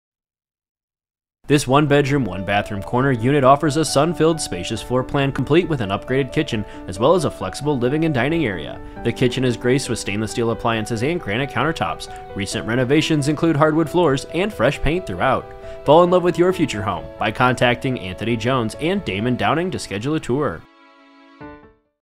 1439 Euclid Street NW Unit #306 Washington, DC | ColdwellBankerHomes.com
1439 Euclid Street NW Unit #306, Washington, DC

Welcome to Euclid Manor! This 1 Bedroom, 1 Bath corner-unit in the heart of Columbia Heights is a must see! Unit #306 offers a sun-filled, spacious floor plan complete with upgraded kitchen, flexible living/dining area. The kitchen is graced with stainless-steel appliances, granite countertops, new